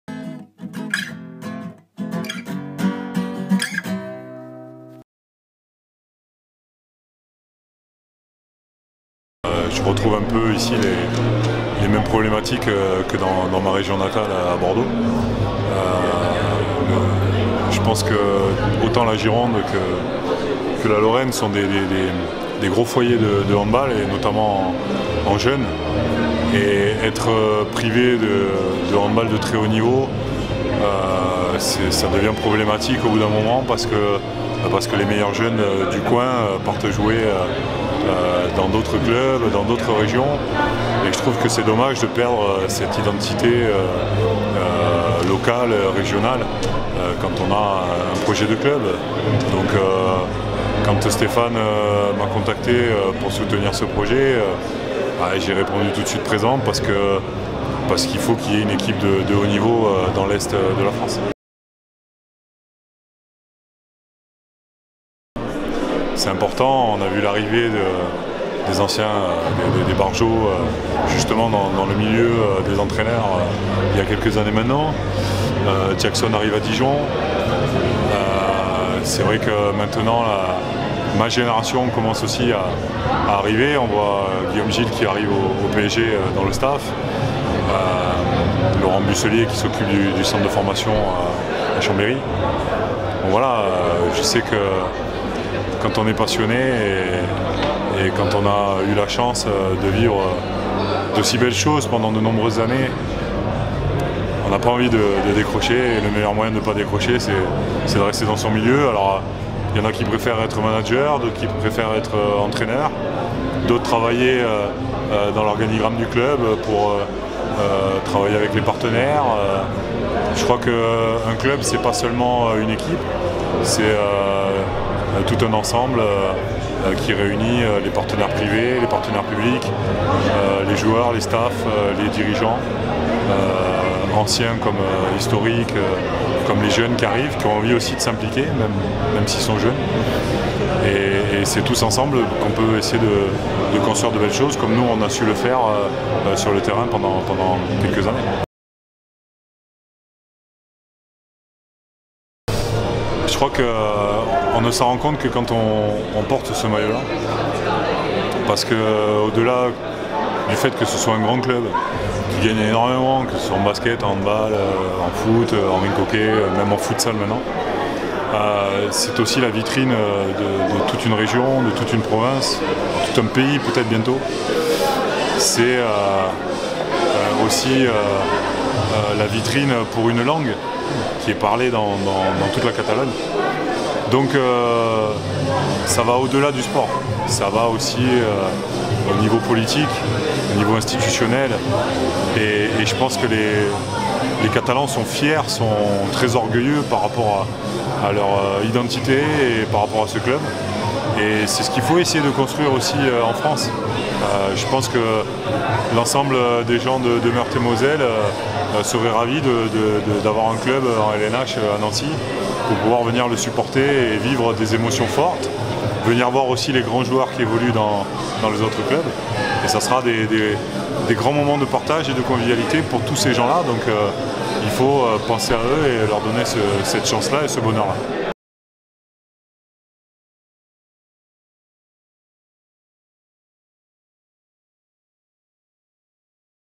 Jérôme Fernandez explique son soutien au GN ASPTT HB
Interview de Jérôme Fernandez qui explique son soutien au projet LNH du GN ASPTT HB, mais aussi les valeurs qu'il souhaite transmettre avec comme exemple le Barça...